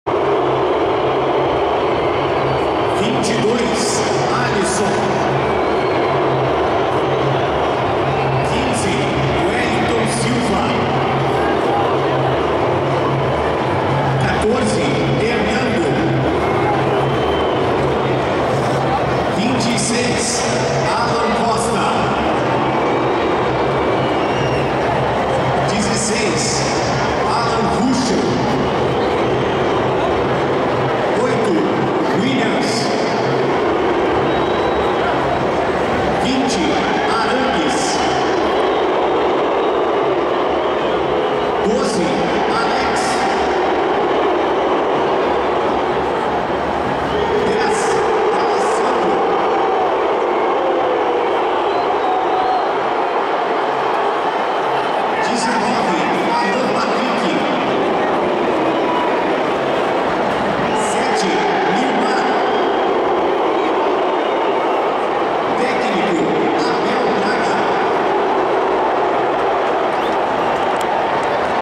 Grêmio Vs Inter - Escalação do inter
FICHA TÉCNICA
GRÊMIO 4 X 1 INTERNACIONAL

Local: Arena do Grêmio, em Porto Alegre (RS)
Data: 9 de novembro de 2014, domingo
Horário: 17 horas (de Brasília)
Árbitro: Luis Flavio de Oliveira (SP)
Assistentes: Emerson Augusto de Carvalho (Fifa-SP) e Marco Luiz Augusto (SP)
Assistentes adicionais: Francisco de Paula dos Santos Silva Neto e Marcio Brum Coruja (ambos do RS)
Cartões amarelos: Geromel e Alan Ruiz (Grêmio); Willians, Rafael Moura e Aránguiz (Inter)

Gols:
GRÊMIO: Luan, aos 27 minutos do primeiro tempo; Ramiro, aos três, e Alan Ruiz, aos 32 e aos 36 minutos do segundo tempo
INTER: Rafael Moura, aos 15 minutos do segundo tempo

GRÊMIO: Marcelo Grohe; Pará, Geromel, Rhodolfo e Zé Roberto; Walace, Fellipe Bastos, Ramiro (Giuliano), Luan (Alan Ruiz (Matheus Biteco)) e Dudu; Barcos
Técnico: Felipão

INTERNACIONAL: Alisson; Wellington Silva, Alan Costa, Ernando e Alan Ruschel; Willians, Aránguiz, Alan Patrick (Rafael Moura), D'Alessandro e Alex (Valdivia); Nilmar (Taiberson)
Técnico: Abel Braga